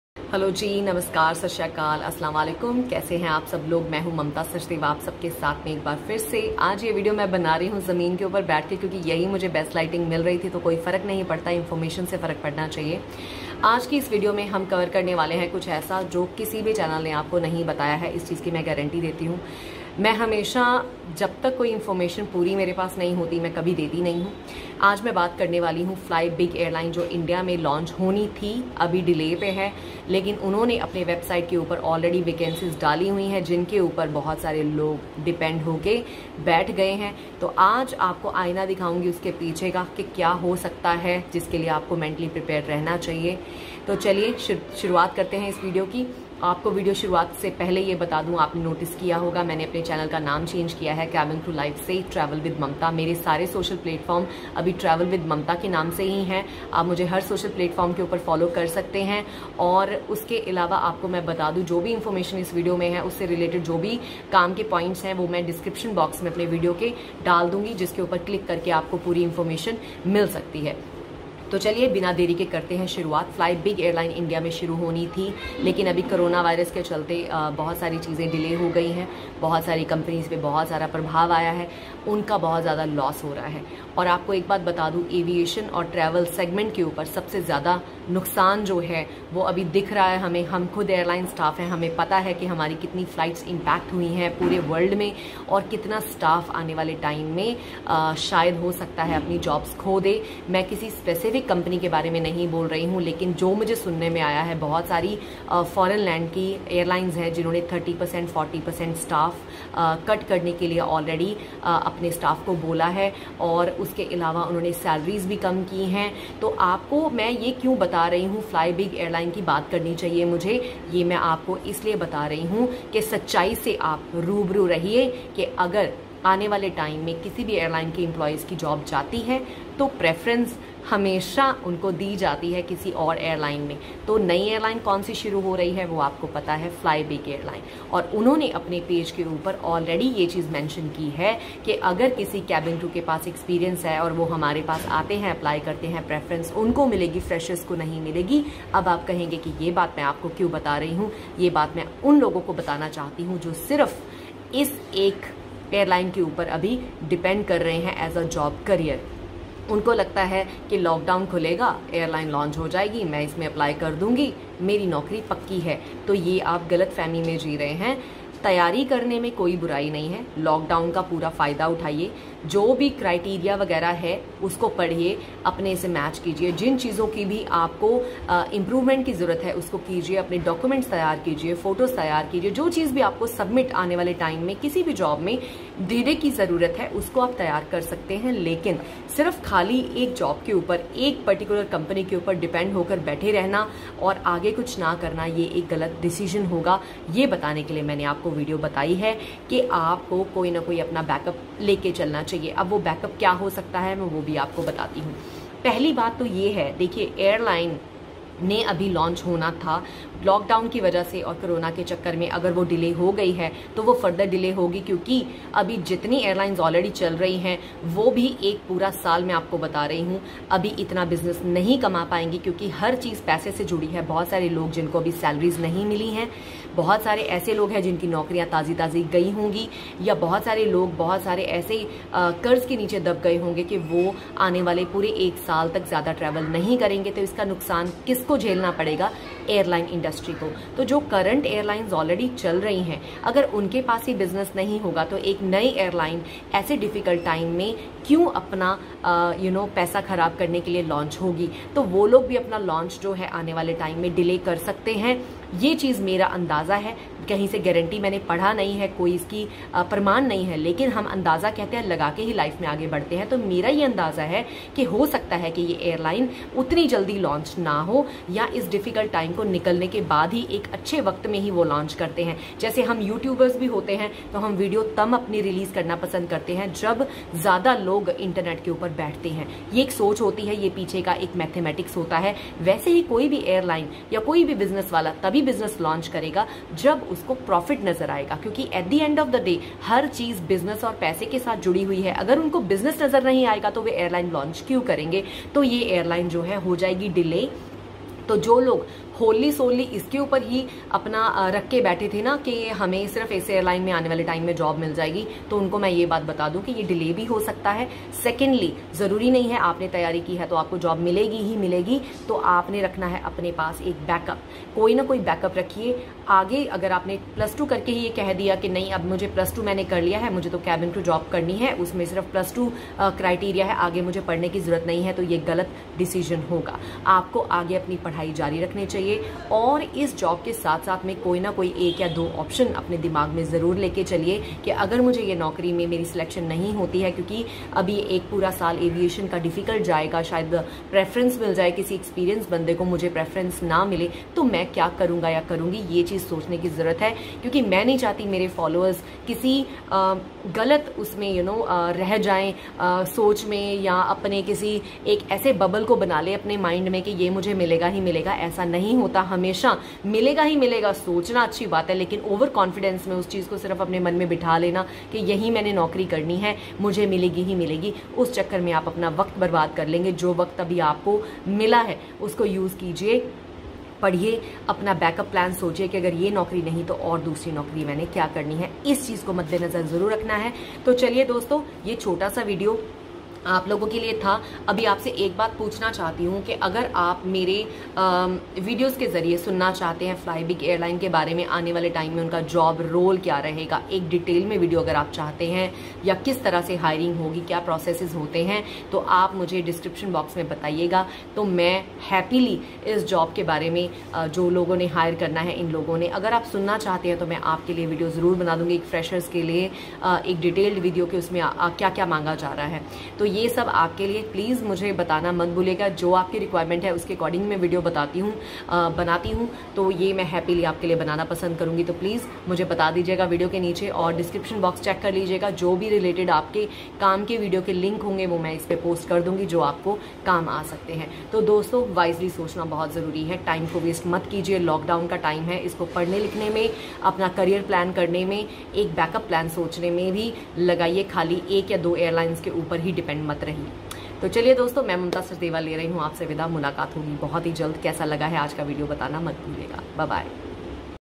Flybig Airline Reality ? New Airline Hiring Cabin Crew/ Air hostess in India,Jobs 2020
Flybig is a new Airlines going to start in India, Due to lockdown it is delayed. Here is the last Statement provided by the Airline Management.
“We are all set to launch our own airline FlyBig by the first week of April. ... He said the promoters are pumping in $50 million into the airline, which will be based out of Indore. “We plan to launch our first flight from Indore to Gondia and through Gondia connect Indore to Hyderabad"